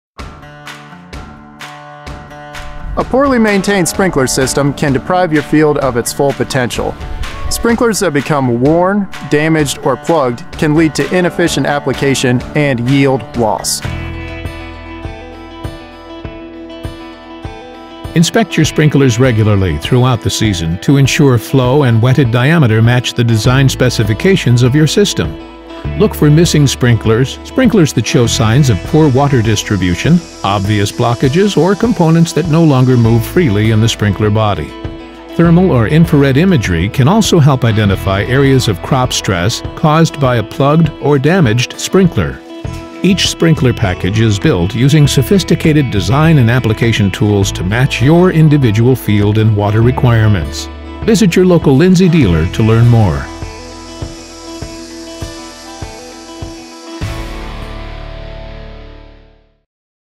Zimmatic Tip: Sprinkler Maintenance
A poorly maintained sprinkler system can deprive your field of its full potential, but a simple visual inspection can improve efficiency and application uniformity. Throughout the season, check for sprinklers that are damaged, worn or plugged to ensure flow and wetted diameter match the design specs of your system. Your sprinkler package may need modifications if you're changing crop types or if there's varying pressure or volume from your water source.